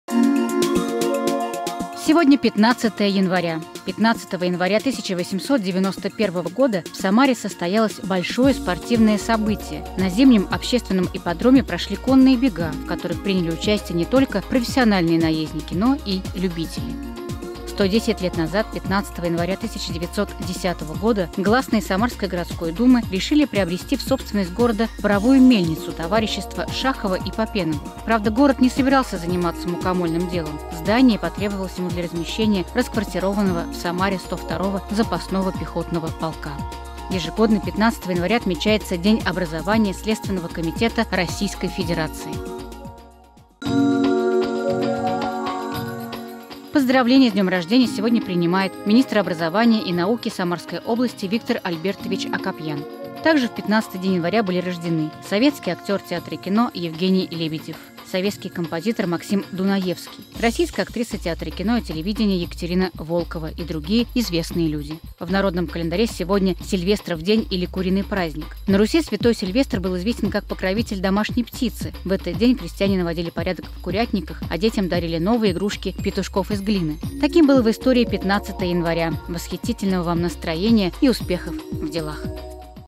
Сильвестров день. Календарь губернии от 15 января
В этот раз оно продлилось больше часа. Участие в нём приняли не только парламентарии, но и представители других ветвей власти, а также руководители регионов. Глава государства обозначил ключевые направления развития страны. Одна из главных тем - повышение уровня доходов населения и связанная с этим аспектом демографическая ситуация.
По инициативе Президента, маткапитал с 1 января 2020 года будет выплачиваться уже при рождении первенца и составит больше 460 тысяч рублей. Предусмотрены и другие формы поддержки семей с детьми. Подробности далее.